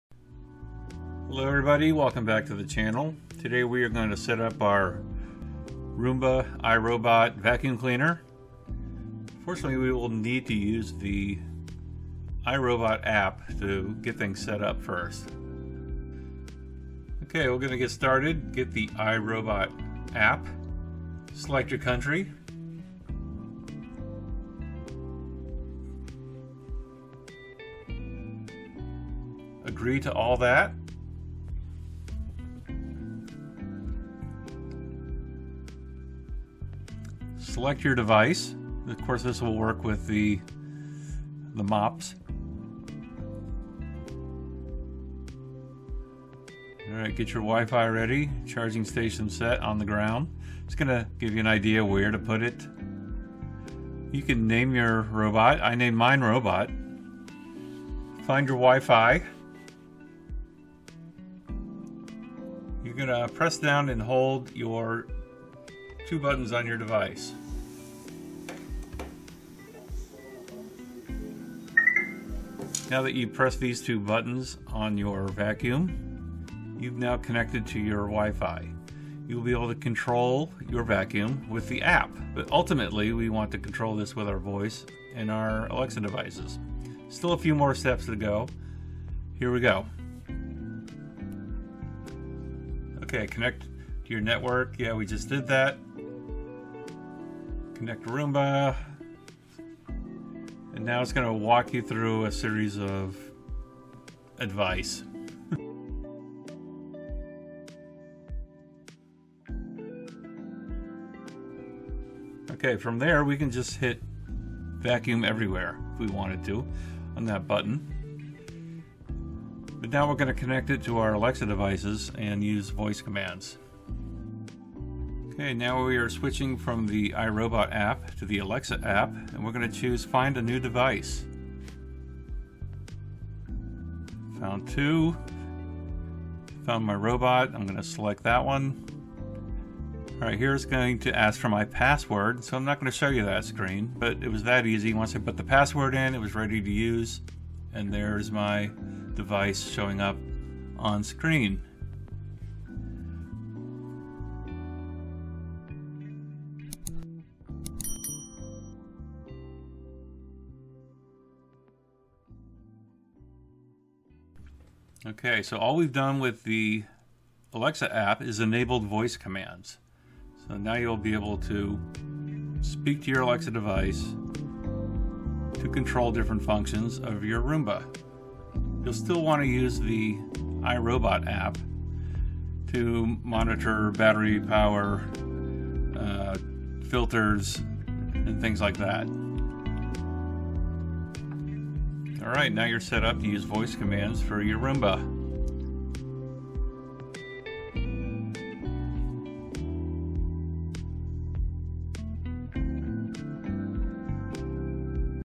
Activate Alexa voice Commands for your Roomba iRobot
Activate voice commands for your Roomba iRobot device when you connect to your Alexa Smart home,  In this video we will walk you through the process with on screen shots of your iRobot and Alexa apps.  You will need your Roomba, Alexa app, Alexa speaker, I Robot app.

Echo Dot (3rd Gen) - Smart speaker